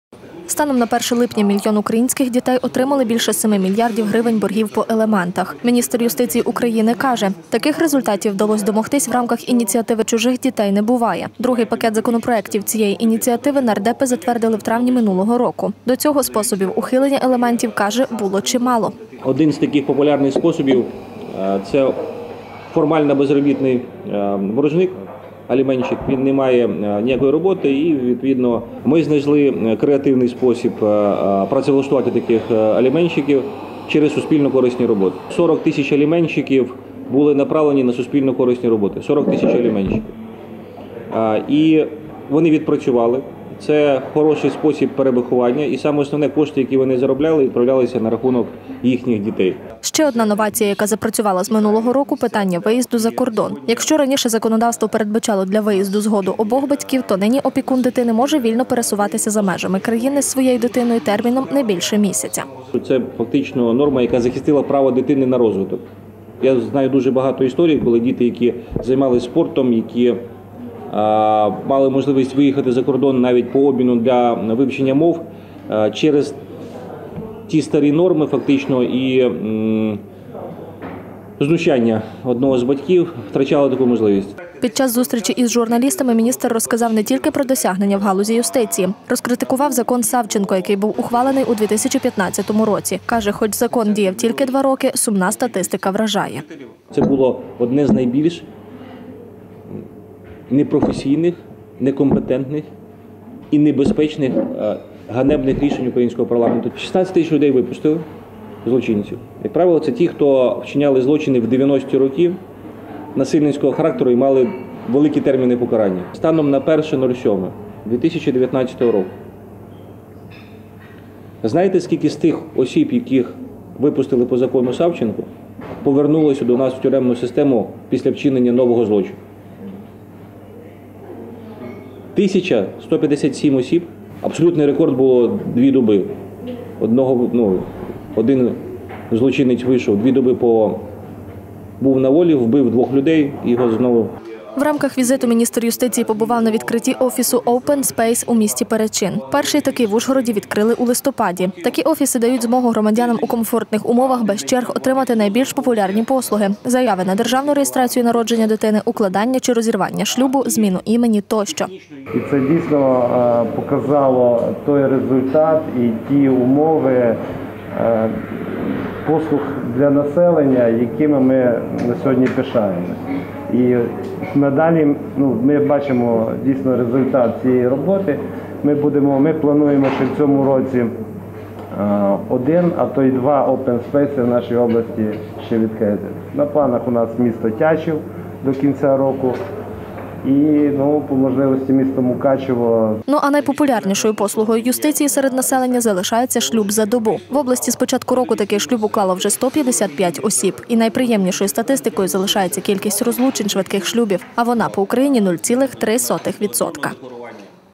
Зустріч з міністром
На Закарпатті з робочим візитом перебував міністр юстиції України Павло Петренко. Приїхав, аби відкрити черговий OpenSpace, здійснити передачу конфіскованих авто на благодійні потреби. Між тим сходив на каву і з журналістами області. Розказав медійниками про досягнення в галузі юстиції, а ще розкритикував «закон Савченко».